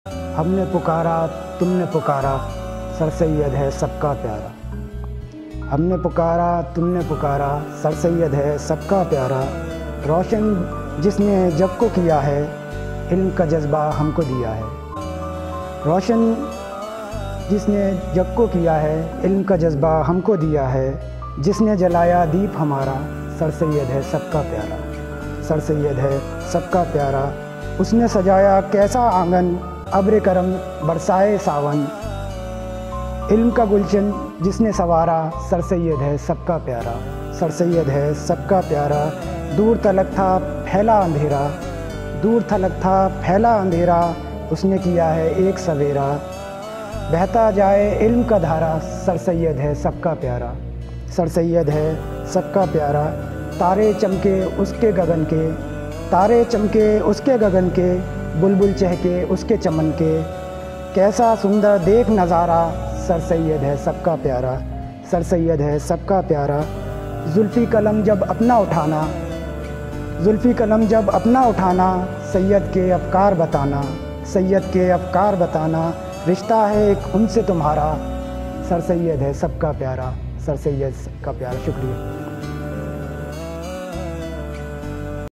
Sir Syed Hai Sbka Pyara ❤️ || Zulfi || Poetry
A Beautiful poetry recitation by Zulfiqar Khan 'Zulfi' for  lovers of Sir Syed Ahmad Khan. 💚